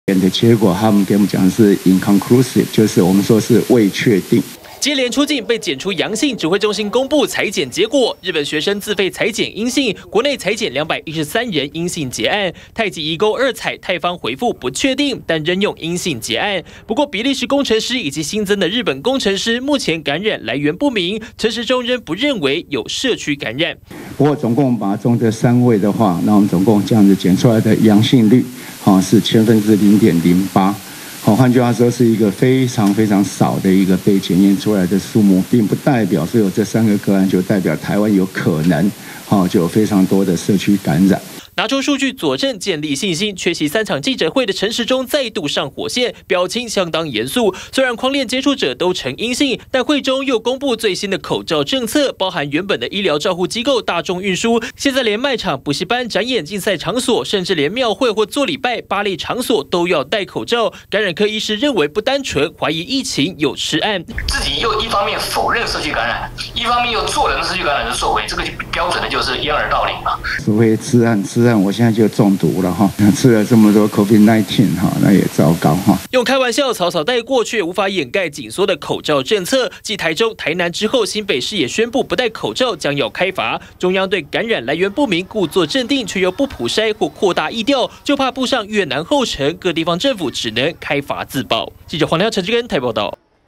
20200806中天新聞 女學生翻版？日籍工程師來台工作返國後確診
繼日籍女學生之後，又有日籍工程師疑似在台染疫了嗎？一名三十多歲日本男性，六月中旬來台工作，八月一號搭機返回日本後，卻在機場採檢陽性確診，陳時中親上火線，強調出境篩檢呈陽性，只有千分之0.08，但另一方面，卻又明列8處場所務必戴口罩，讓感染科醫師直呼是掩耳盜鈴。